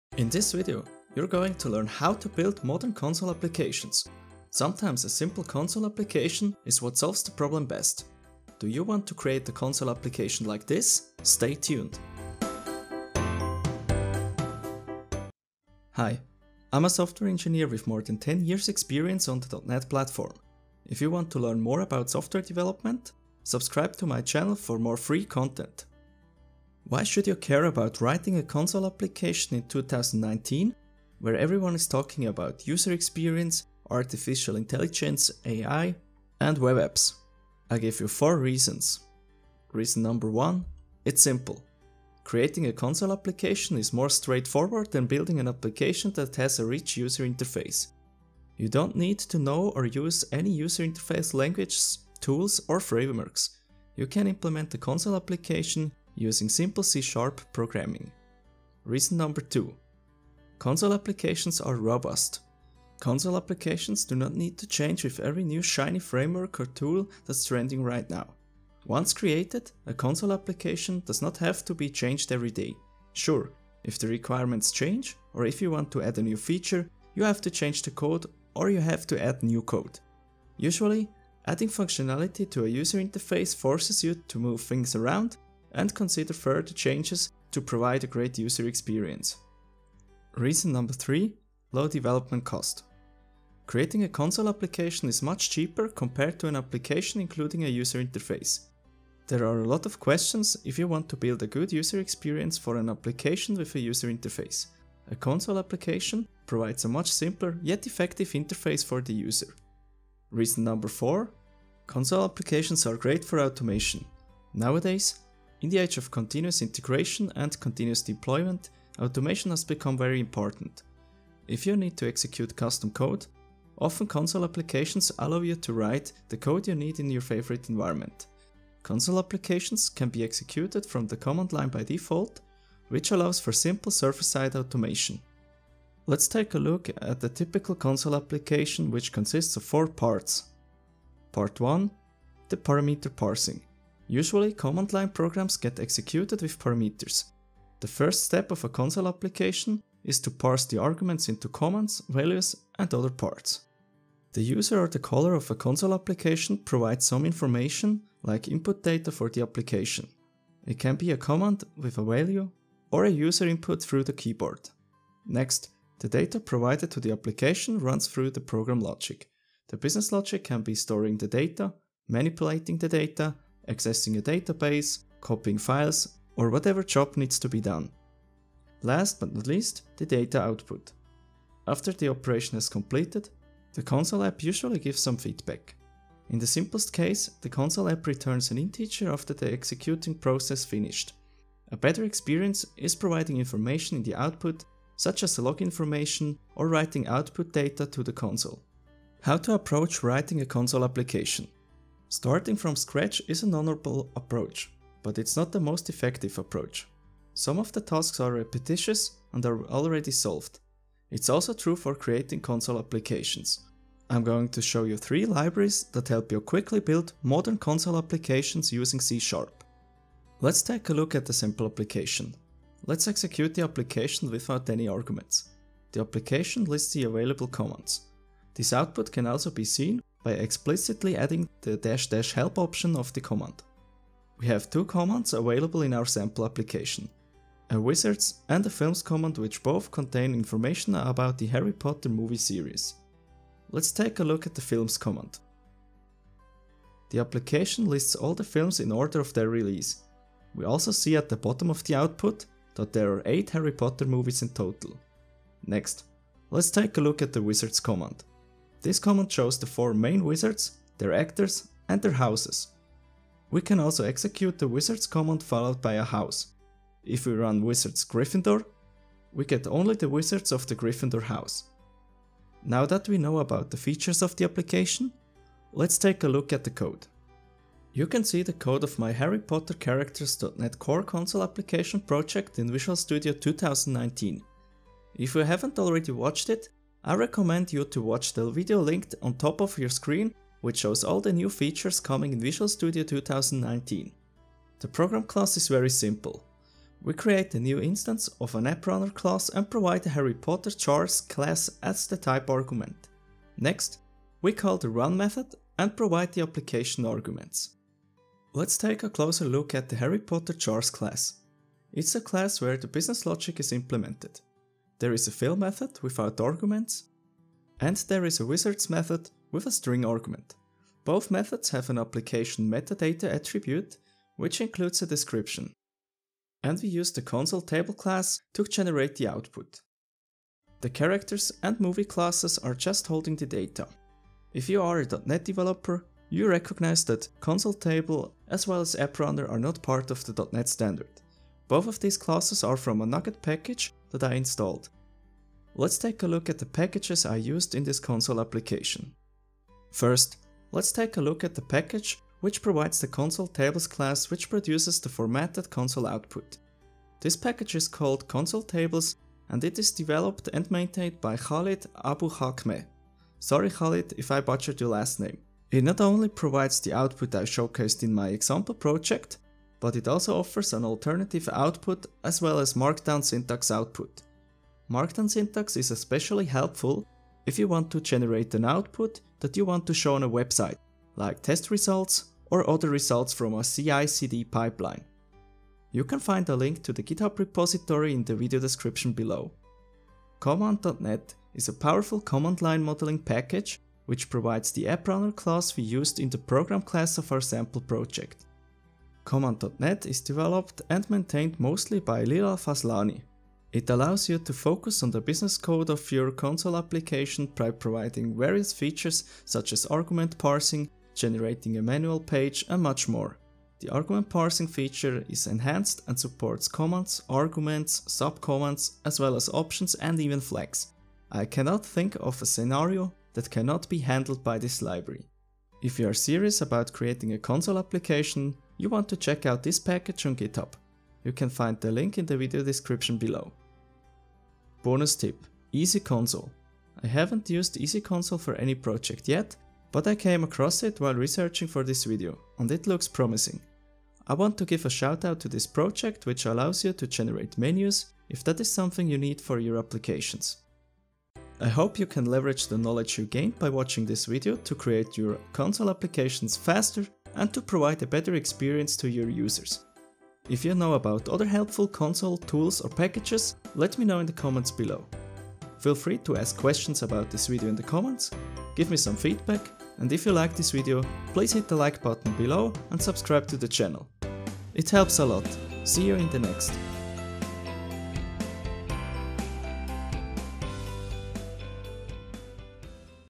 How to Create Modern C# Console Applications
In this video, you're going to learn how to build modern console applications. Sometimes a simple console application is what solves the problem best. Do you want to create a console app like this?

Why should you care about writing a console application in 2019 where everyone is talking about user experience, artificial intelligence (AI) and web apps? I'll give you four reasons:

- Simplicity
- Robustness
- Low development cost
- Great for automation

How to approach writing a console application? Starting from scratch is an honorable approach, but it's not the most efficient approach. Some of the tasks are repetitious and are already solved. It's also true for creating console applications.

In this video, I show you three NuGet packages which help you to create modern C# console applications:

- ConsoleTables
- CommandDotNet
- EasyConsole

00:00 Introduction
00:27 Why Console Applications?
02:16 4 Parts of a Console Application
03:19 Don't start from scratch
04:38 Code for the sample application
05:57 ConsoleTables package
06:29 CommandDotNet package
07:15 EasyConsole package
07:33 Conclusion